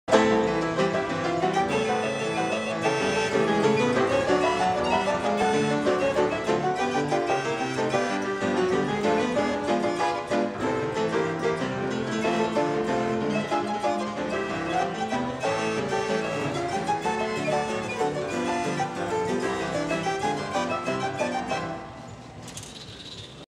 Montage of Musee Mechanique Items
From the ARCADE Documentary project a small collection of shots taken at the beautiful Musee Mechanique in San Francisco